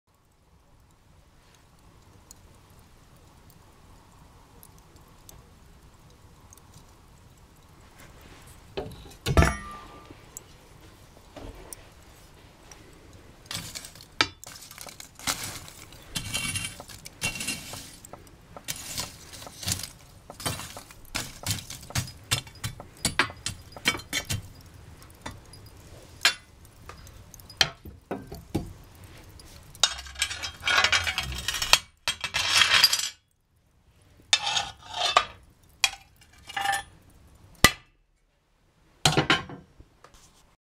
Seconds of stove embers.mov
Birch wood stove, final stoking of charcoals and closing after 2 hours burning. Next heating in 22 hours.